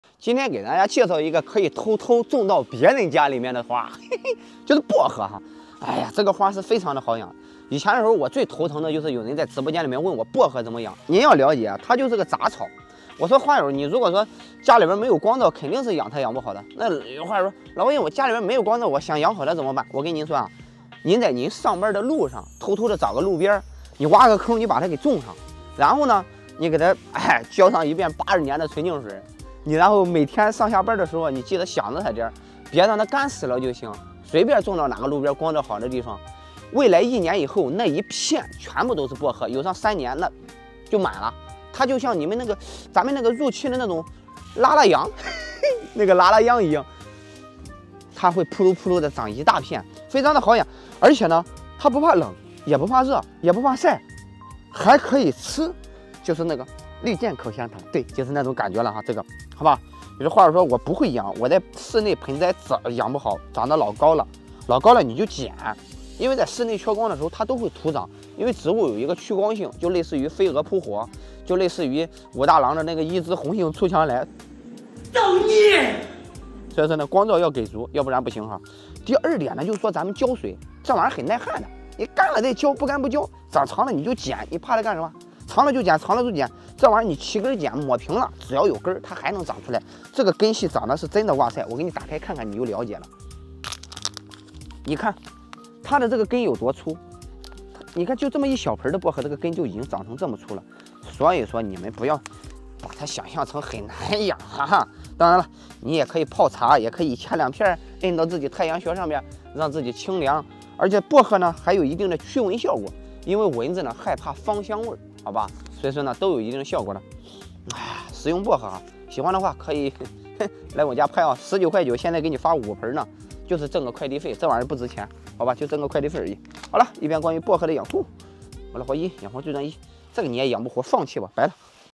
【老花医植物推荐】一种可以偷偷种在别人家的花？养护起来极其简单？薄荷养护分享 Tips for Mint grow and care.
薄荷养护起来极其简单，薄荷养护分享！
It is very simple for planting Mint . Tips for Mint grow and care.